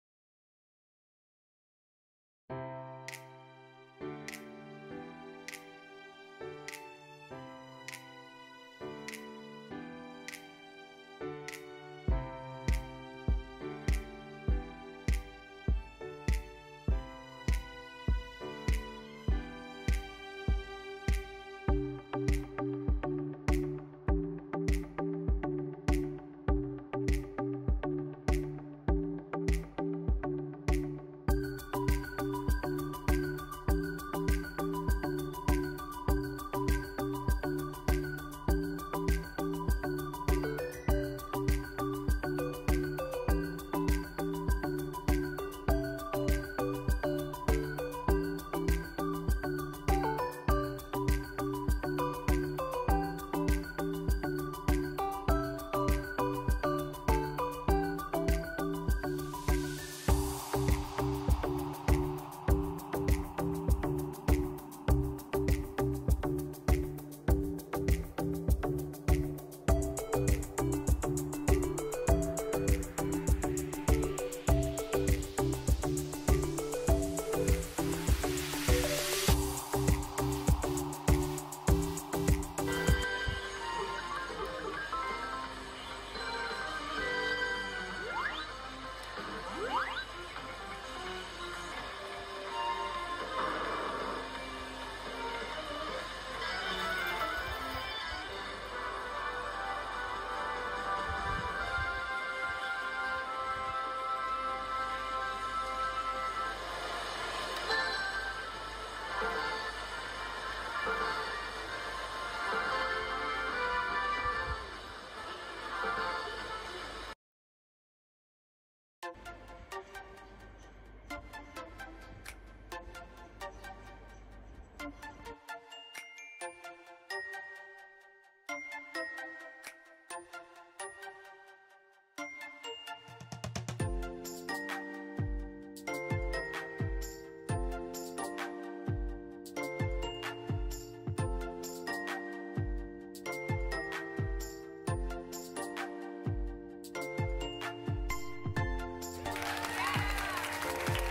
パチスロわずか１日で１２０万円✨勝利〜恐るべし初代GOD⁉️
くだらないボツネタです！